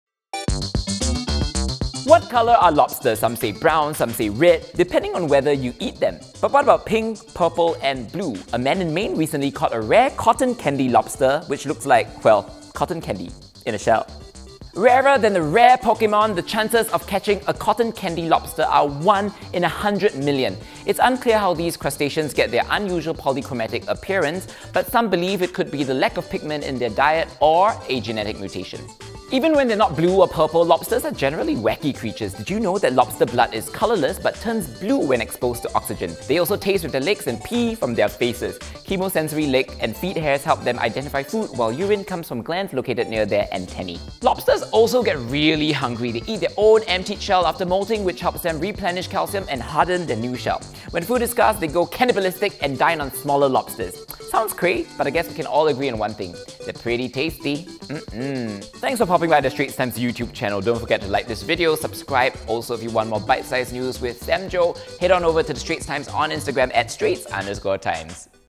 Cotton Candy Lobsters & Weird Lobster Facts | Bite-Size News with Sam Jo | The Straits Times
Lobsters are usually brown, but the cotton candy lobster has a shell that shimmers in pink, purple and blue. The chance of catching one of these crustaceans is 1 in 100 million. ST correspondent Yeo Sam Jo shares more about this unusual creature, as well as weird trivia that might make you look at your lobster bisque differently.

For more Bite-Size News with Sam Jo, follow The Straits Times' on Instagram @straits_times